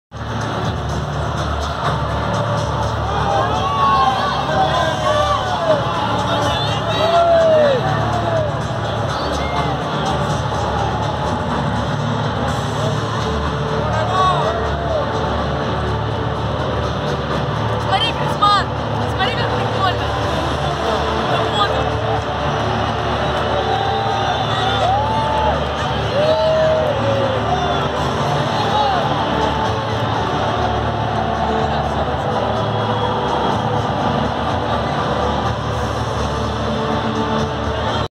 Atletico Madrid Players' Epic Entrance to the Pitch!
Atletico Madrid players entering the pitch before the game Atletico Madrid - Monaco

Get ready for an unforgettable experience as we take you behind the scenes of Atletico Madrid's players' epic entrance to the pitch! 🎉⚽ In this video, witness the electrifying atmosphere as the team prepares to showcase their skills and passion in front of their devoted fans. From the adrenaline of the warm-up to the dramatic moments leading up to kickoff, feel the excitement build as we capture every thrilling detail.

Join us in celebrating the energy, camaraderie, and dedication of Atletico Madrid's players as they walk onto the field, ready to battle it out for glory!